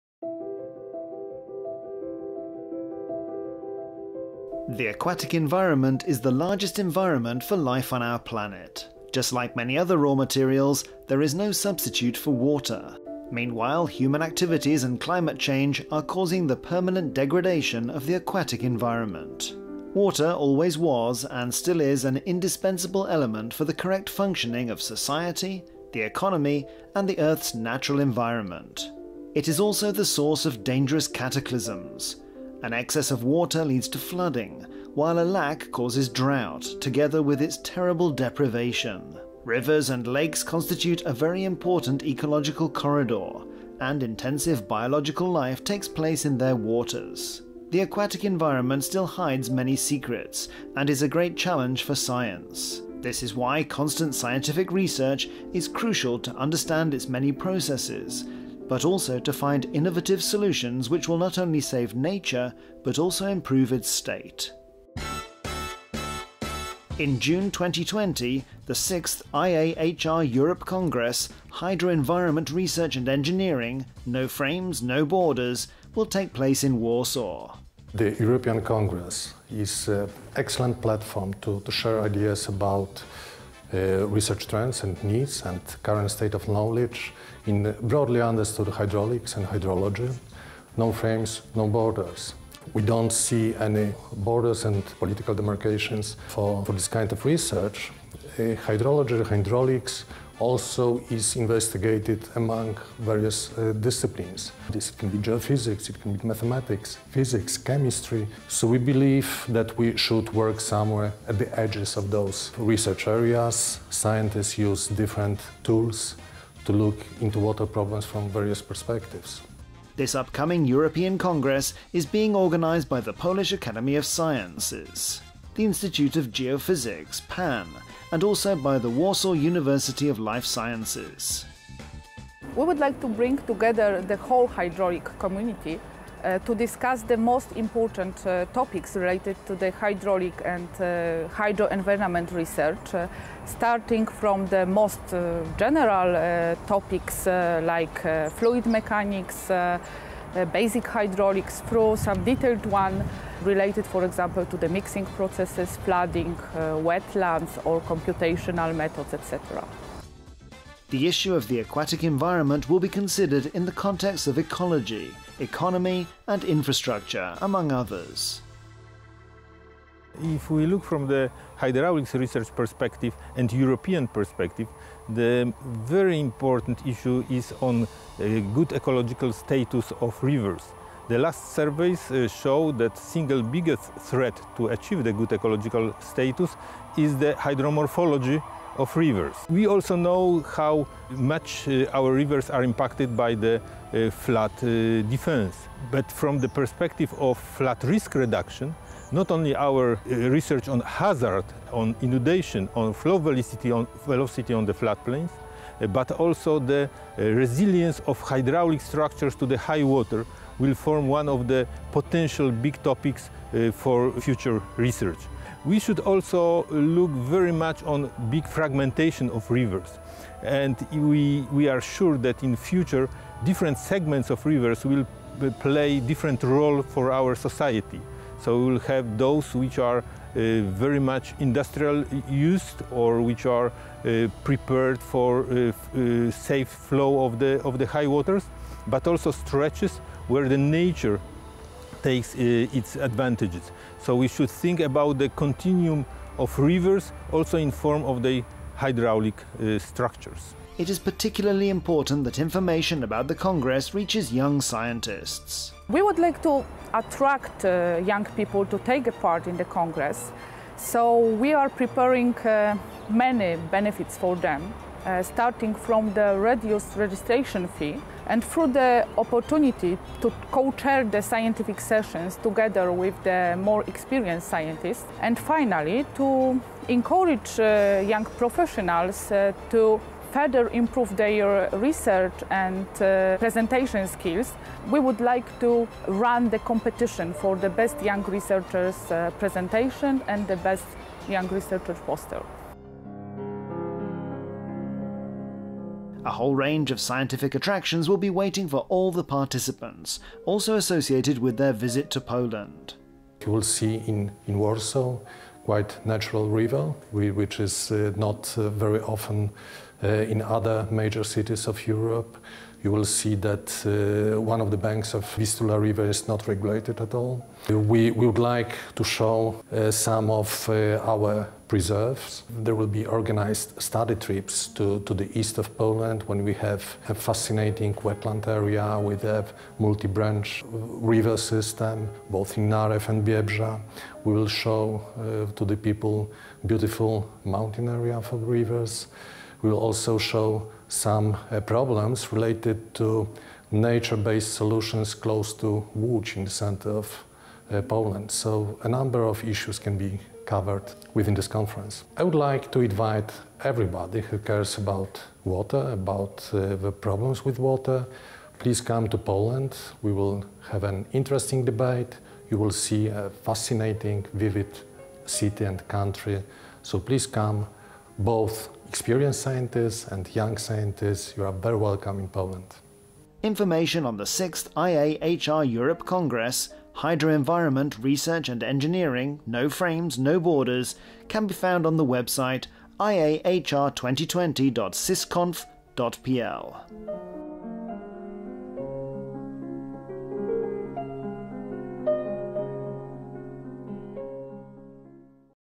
6th IAHR Europe Congress Warsaw 2020 - promotional video
We are pleased to announce the 6th IAHR Europe Congress "Hydro-environment Research and Engineering - No Frames, No Borders", which will be held in Warsaw, Poland, 30 June - 2 July 2020.